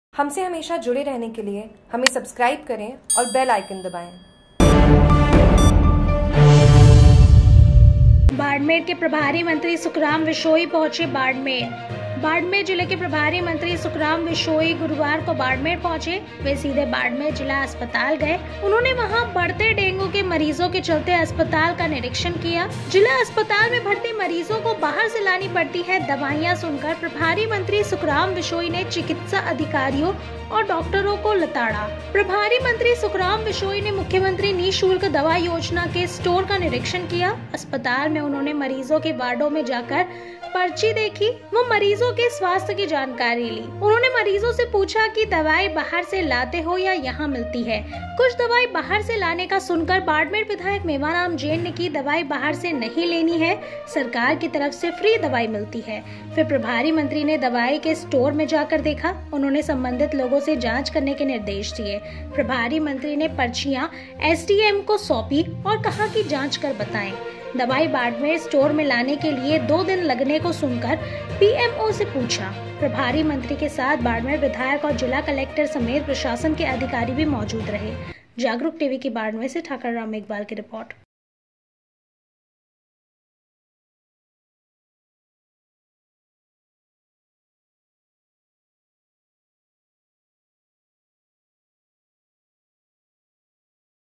प्रभारी मंत्री सुखराम विश्नोई पहुंचे बाड़मेर
बाड़मेर जिले के प्रभारी मंत्री सुखराम विश्नोई गुरुवार को बाड़मेर पहुंचे। वे सीधे बाड़मेर जिला अस्पताल गए। उन्होंने वहां बढ़ते डेंगू के मरीजों के चलते अस्पताल का निरीक्षण किया। जिला अस्पताल में भर्ती मरीजों को बाहर से लानी पड़ती हैं दवाइयां सुनकर प्रभारी मंत्री सुखराम बिश्नोई ने चिकित्सा अधिकारियों और डाक्टरों को लताड़ा।

प्रभारी मंत्री सुखराम विश्नोई ने मुख्यमंत्री नि:शुल्क दवा योजना के स्टोर का निरीक्षण किया। अस्पताल में उन्होंने मरीजों के वार्डों में जाकर पर्ची देखी व मरीजों के स्वास्थ्य की जानकारी ली। उन्होंने  मरीजो से पूछा कि दवाई बाहर से लाते हो या यहां मिलती है। कुछ दवाई बाहर से लाने का सुनकर बाड़मेर विधायक मेवाराम जैन ने  कि दवाई बाहर से नहीं लेनी है। सरकार की तरफ से फ्री दवाई मिलती है। फिर प्रभारी मंत्री ने दवाई के स्टोर में जाकर देखा। उन्होंने संबंधित लोगों से जांच करने के निदेश दिए। प्रभारी मंत्री ने पर्चियां एसडीएम को सौंपी और कहा कि जांच कर बताएं। दवाई बाड़मेर स्टोर में लाने के लिए दो दिन लगने को सुनकर पीएमओ से पूछा। प्रभारी मंत्री के साथ बाड़मेर विधायक और जिला कलेक्टर समेत प्रशासन के अधिकारी भी मौजूद रहे।